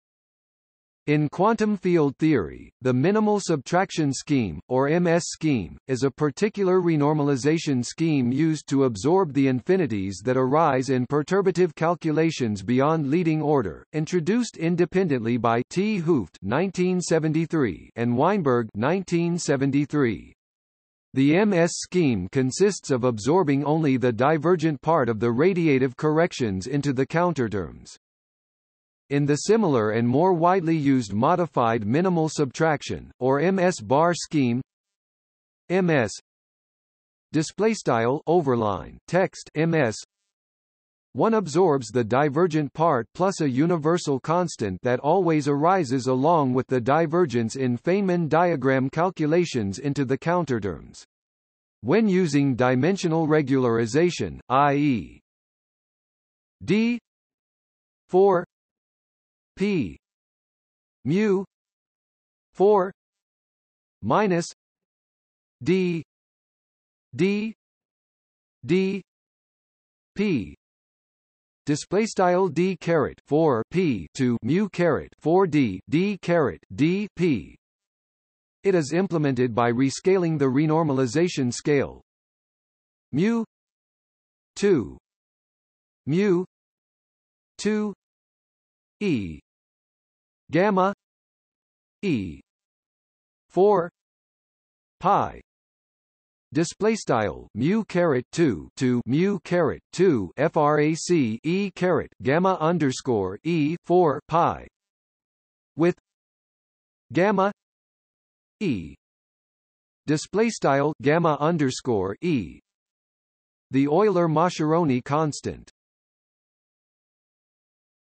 Modified minimal subtraction scheme | Wikipedia audio article
- Socrates

SUMMARY
In quantum field theory, the minimal subtraction scheme, or MS scheme, is a particular renormalization scheme used to absorb the infinities that arise in perturbative calculations beyond leading order, introduced independently by 't Hooft (1973) and Weinberg (1973).  The MS scheme consists of absorbing only the divergent part of the radiative corrections into the counterterms.
In the similar and more widely used modified minimal subtraction, or MS-bar scheme (

    {\displaystyle {\overline {\text{MS}}}}
  ), one absorbs the divergent part plus a universal constant that always arises along with the divergence in Feynman diagram calculations into the counterterms. When using dimensional regularization, i.e.

          d

            4

          μ

            4
            d

          d

            d

        p

    {\displaystyle d^{4}p\to \mu ^{4-d}d^{d}p}
  , it is implemented by rescaling the renormalization scale:

          μ

            2

          μ

            2

              e

                  γ

                    E

              4
              π

    {\displaystyle \mu ^{2}\to \mu ^{2}{\frac {e^{\gamma _{E}}}{4\pi }}}
  , with

          γ

            E

    {\displaystyle \gamma _{E}}
    the Euler–Mascheroni constant.